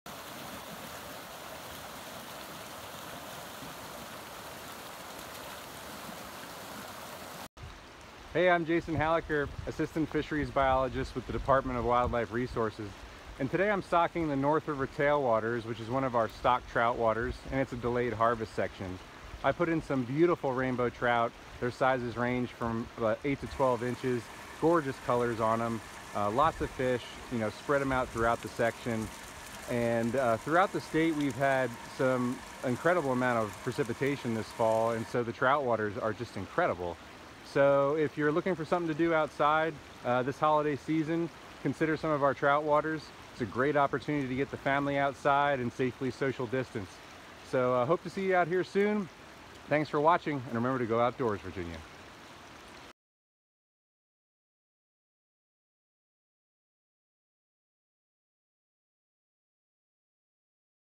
North River Tailwaters
DWR fisheries staff has been stocking trout, and with the recent precipitation, trout waters are excellent. It’s a great time to get outside with the family and do some fishing!